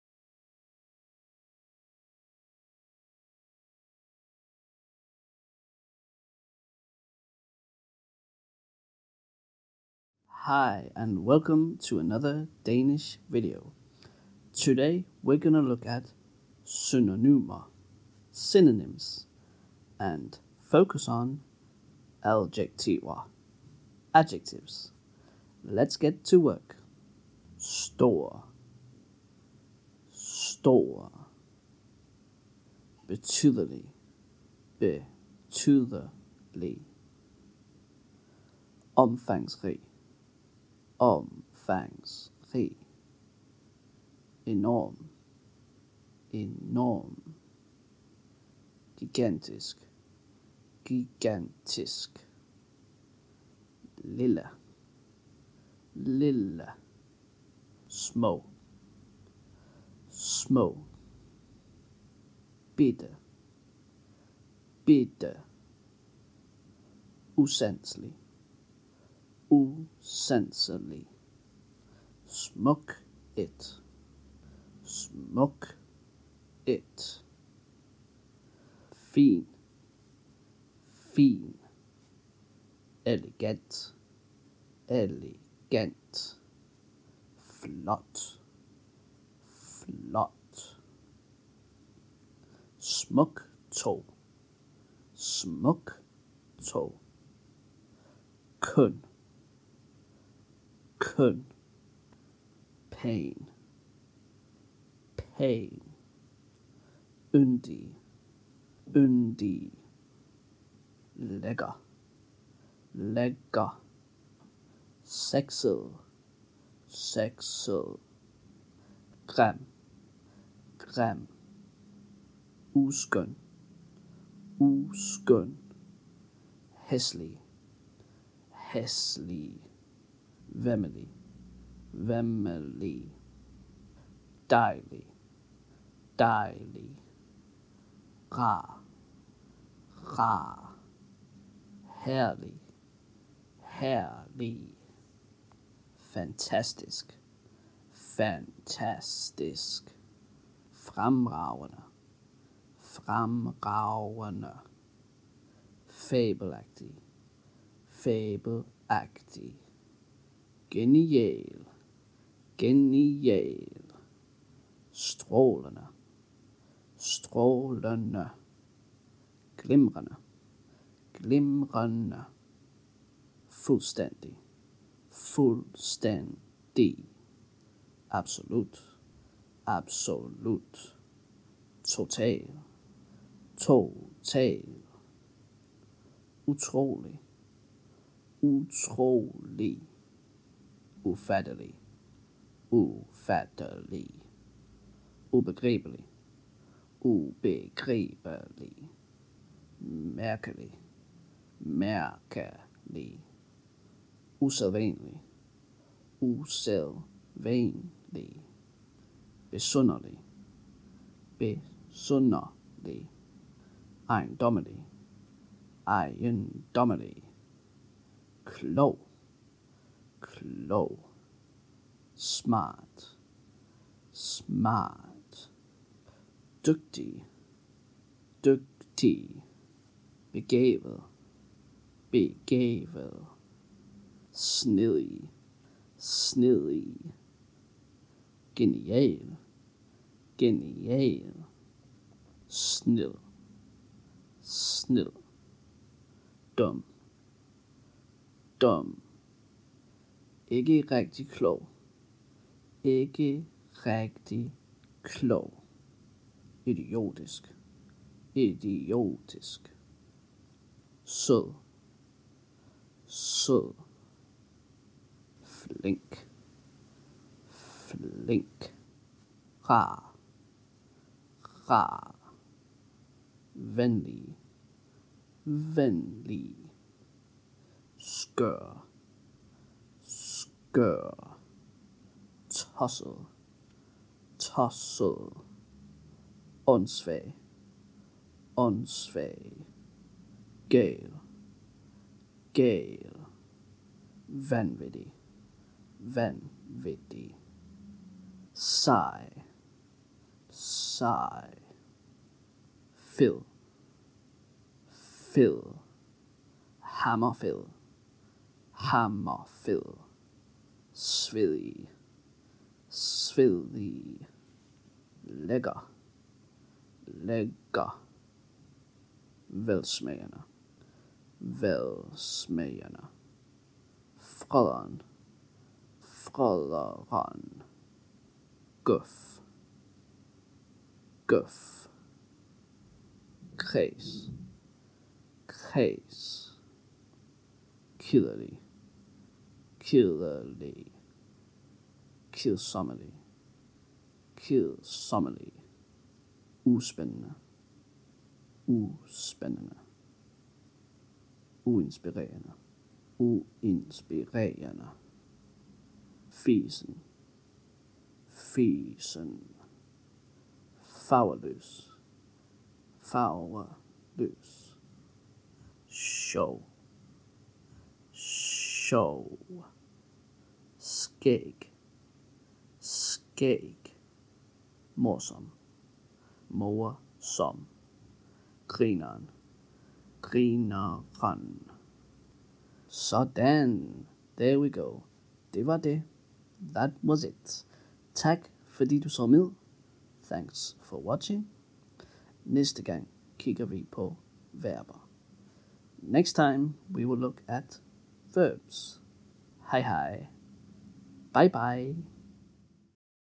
A Taste of Danish Word Choice - Synonyms and Adjectives
Time to get back to work with a taste of Danish word choice, and today: synonyms. Here are some common examples when looking at adjectives.